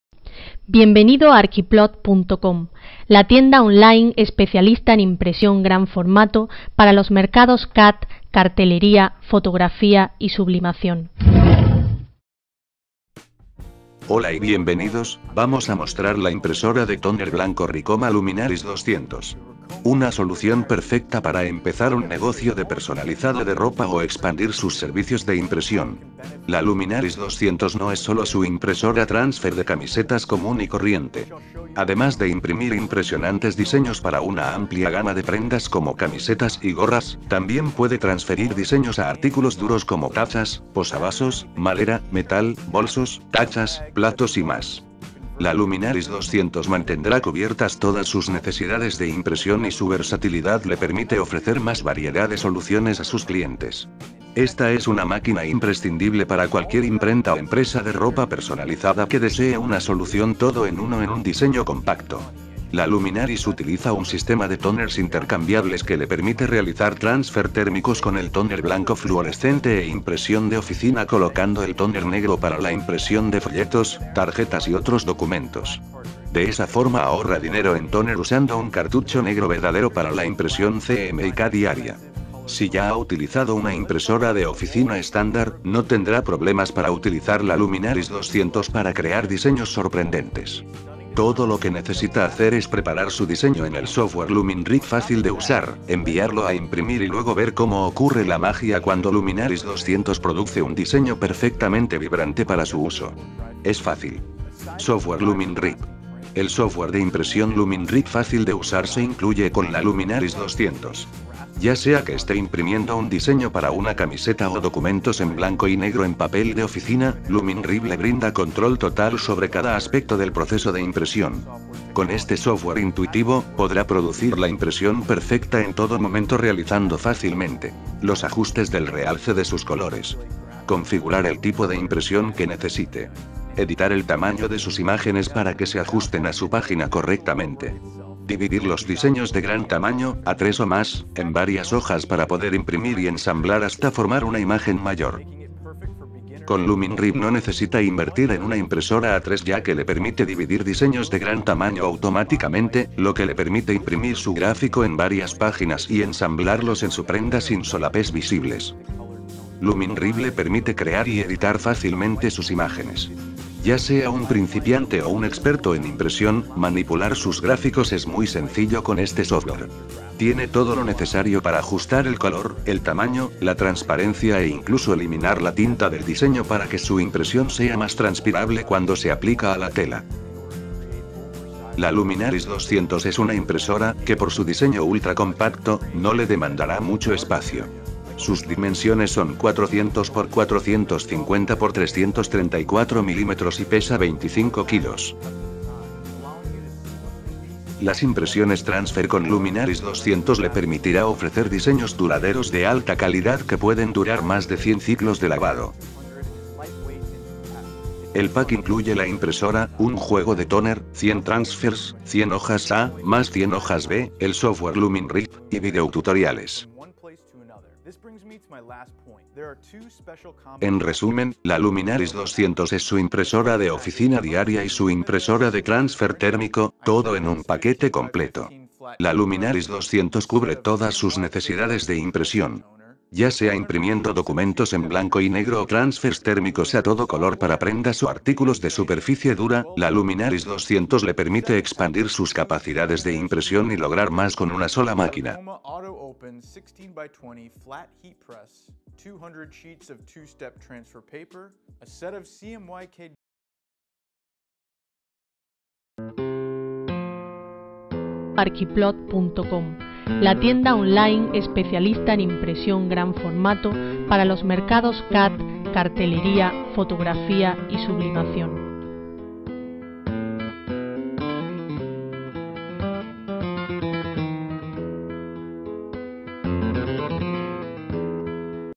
Ricoma Luminaris 200
Impresora láser A4 para transfer No Cut de tóner blanco sobre textil o rígidos e impresión de documentos.
La Luminaris 200 es una de las impresoras de transferencia más versátiles del mercado. Puede imprimir diseños a todo color que se pueden transferir a algodón, poliéster, prendas oscuras, prendas claras e incluso superficies duras. Esto le permite crear diseños sorprendentes en una amplia gama de prendas y artículos de uso común, como camisetas, sudaderas, chaquetas, gorras, delantales, bolsos, menús, tazas y más. Puede transferir cualquier obra de arte a telas claras y oscuras cuando se combina con el papel de transferencia de dos pasos para obtener colores vivos, blancos brillantes y negros verdaderos. Y con su tóner blanco fluorescente, la Luminaris 200 puede imprimir diseños brillantes y vibrantes sin dejar manchas de tinta. Eso significa que no tendrá que perder tiempo limpiando después de completar un trabajo.